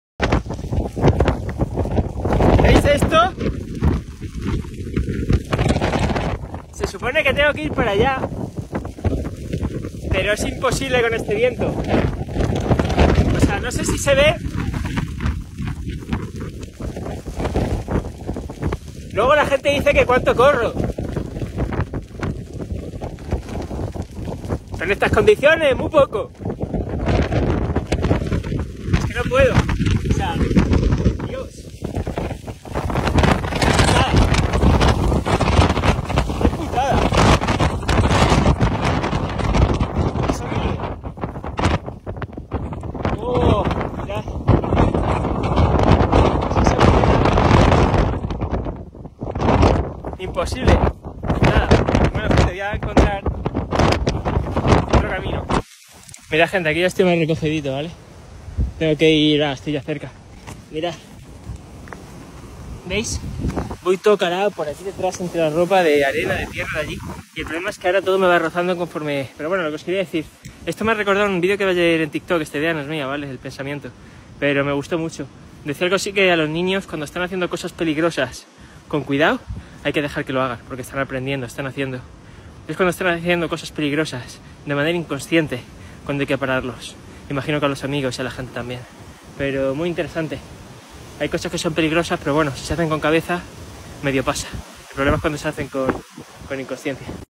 IRL_ Trail Running Risk Management
La llama quema.. pero también calienta..

La gestión del riesgo es importante en el trail running.. al final, todos hemos tenido y volveremos a tener problemas.. forma parte del juego ; quiero decir.. de la vida misma, vaya, no te parece !?